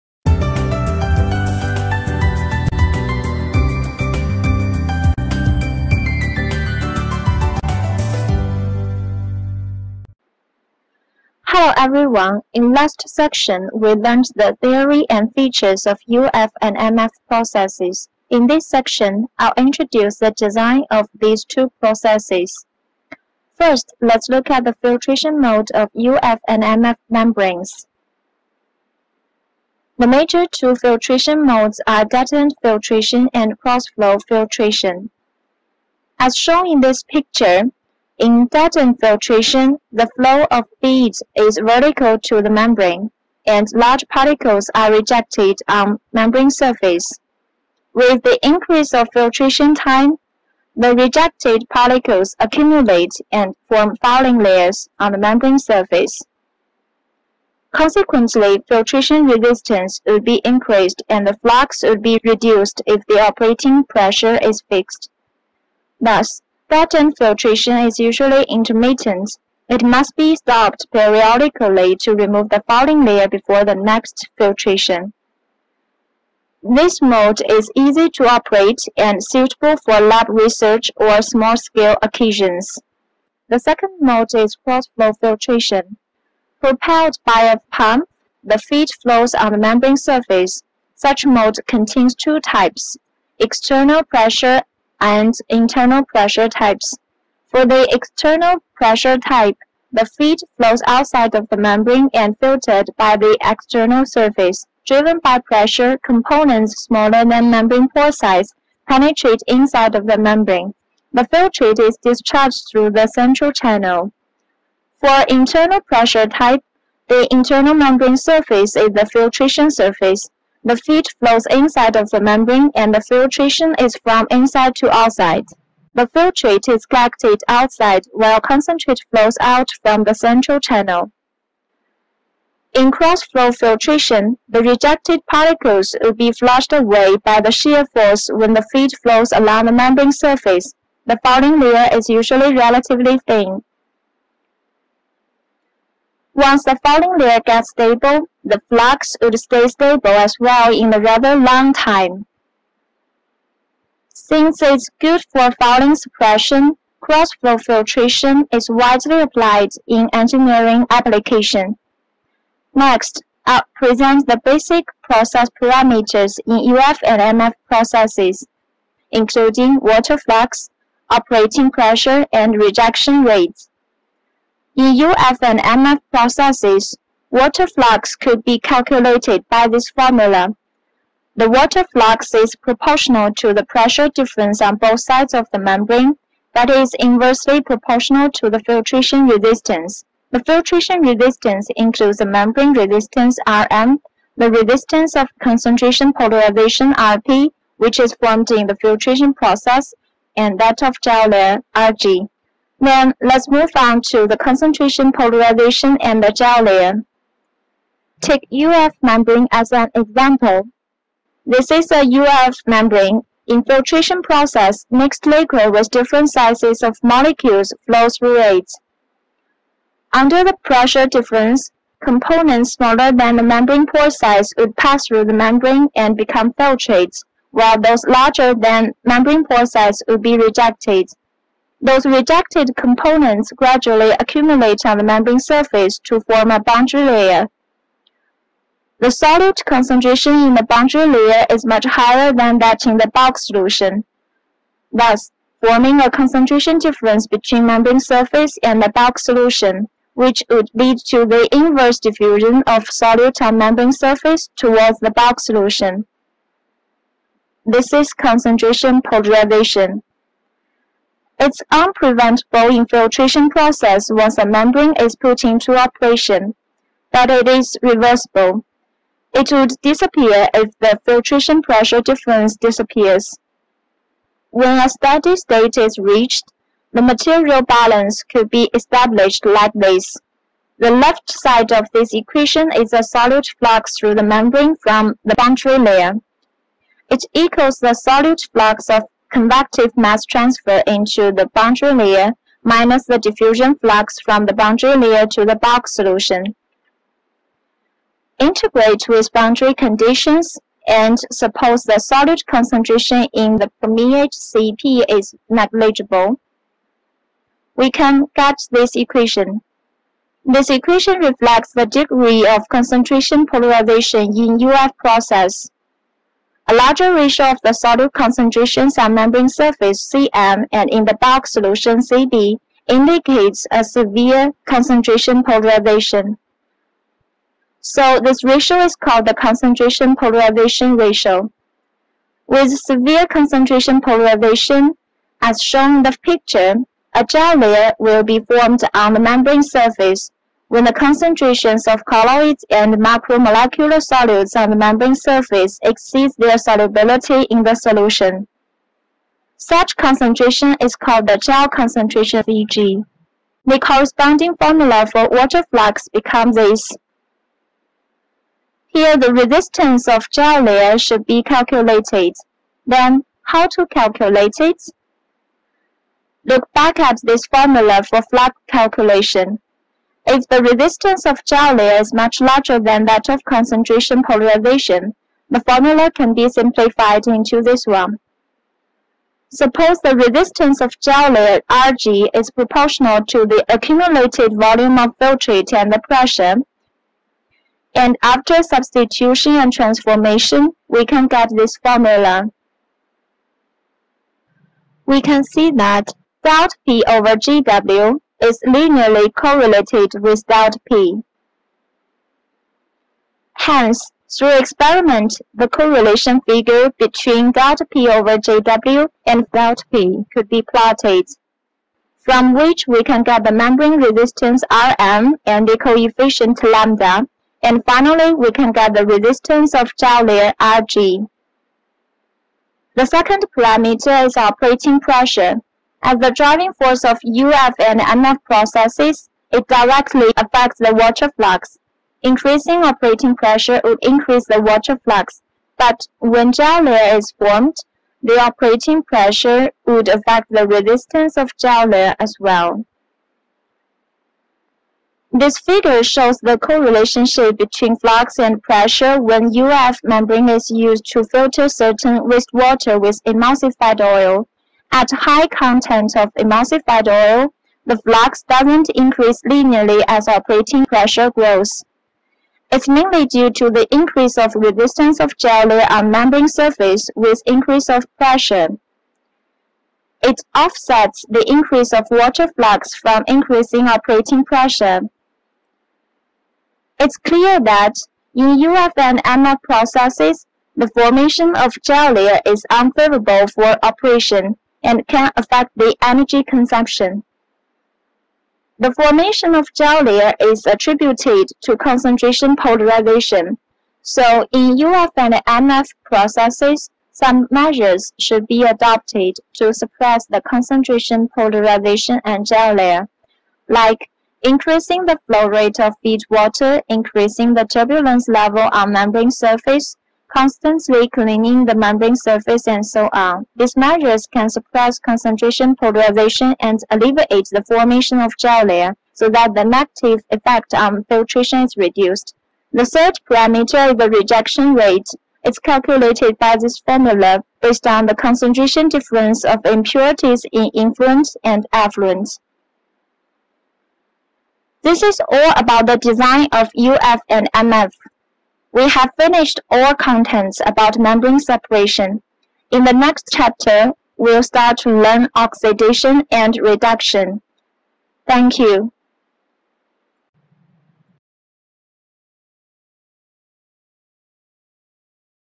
7 8 Design of ultrafiltration and microfiltration process YouTube
المحاضرةالثانية من محاضرات  membrane filtration
من جامعة تسينغ وا  الصينية

تمنياتي بالتوفيق